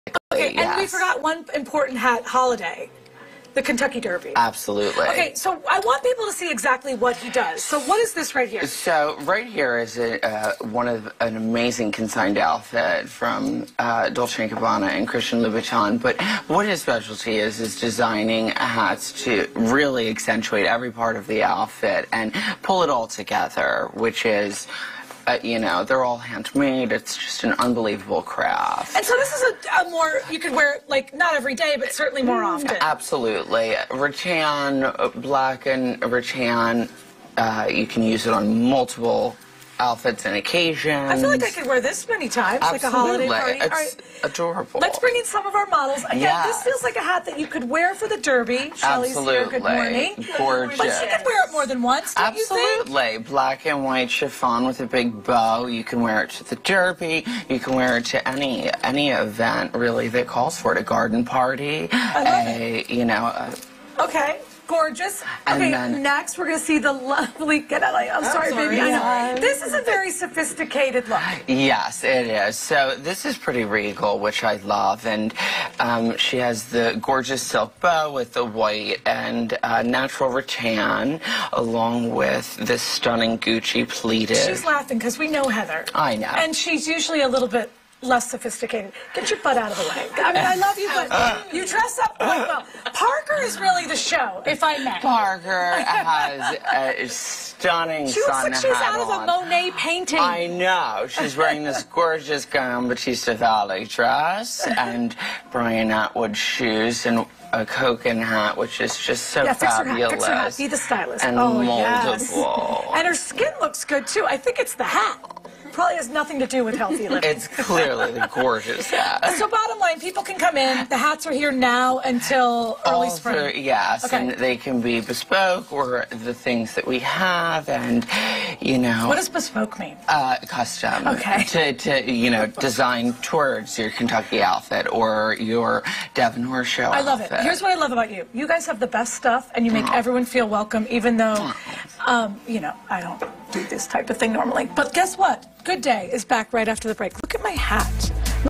Local designer makes hats worn by actresses in 'Sex and the City' actresses - Rachelle Consignment
FOX 29's Jenn Frederick is at Rachelle Boutique and Designer Consignment to talk fashion and fabulous hats that can pull any outfit together!

Video Credit: Fox29 Philadelphia